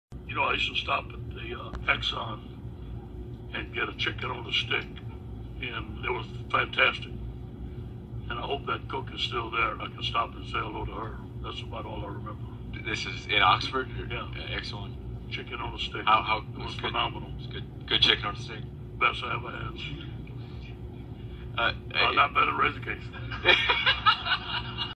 Coach Orgeron Misses Chicken-on-a-Stick
Coach O doesn't have many fond memories of Oxford except for the delicious chicken-on-a-stick at Chevron.